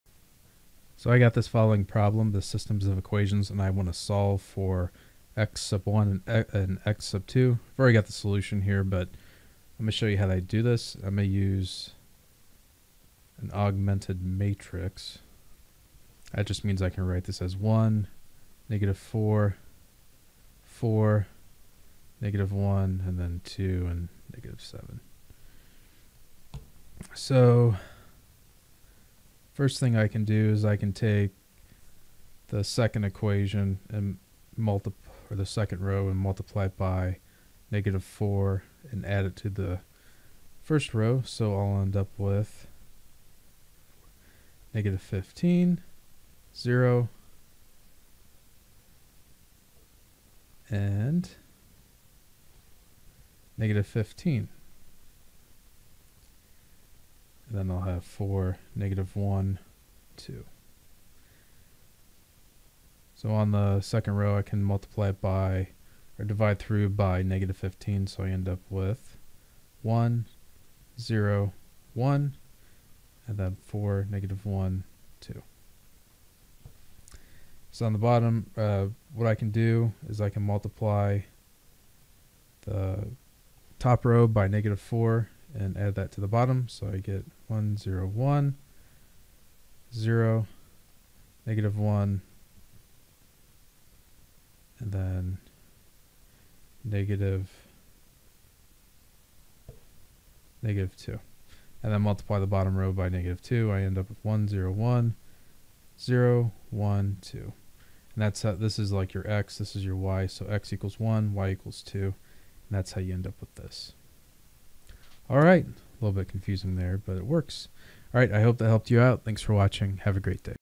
Solve the system of equations using augmented matrix methods. x1 - 4x2 = -7 4x1 - x2 = 2
Solve the system of equations using augmented matrix methods.

x1 - 4x2 = -7
4x1 - x2 = 2